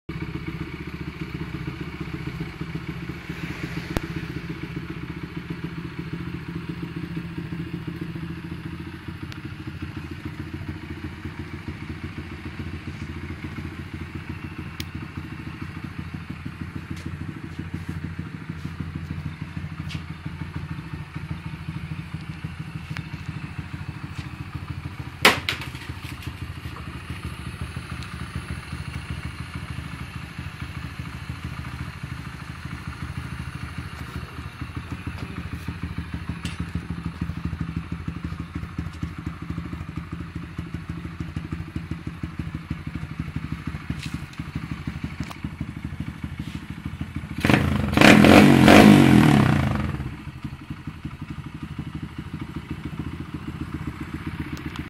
monkey 125cc
my monkey bike with 125cc takegawa bomber exhaust, mikuni vm 26 carb, takegawa oil cooler and Asian Concepts rear swing arm